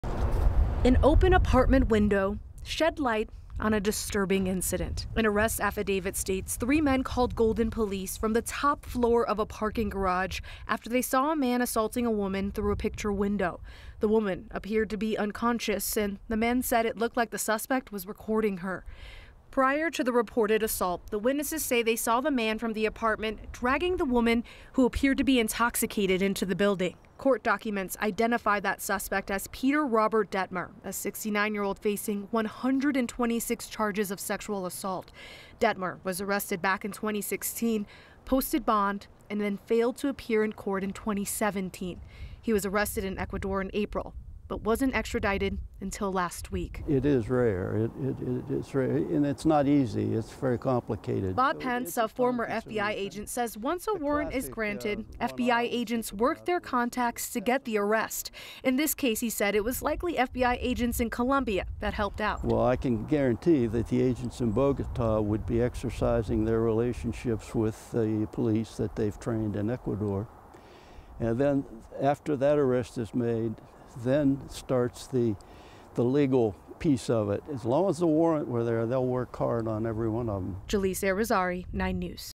Man extradited from Ecuador to Colorado on 126 sex assault charges
Peter Robert Dettmer, 69, is the second person to be extradited from Ecuador to the U.S. in the last 27 years, the First Judicial District Attorney's Office said.

9NEWS (KUSA) is located in Denver, Colorado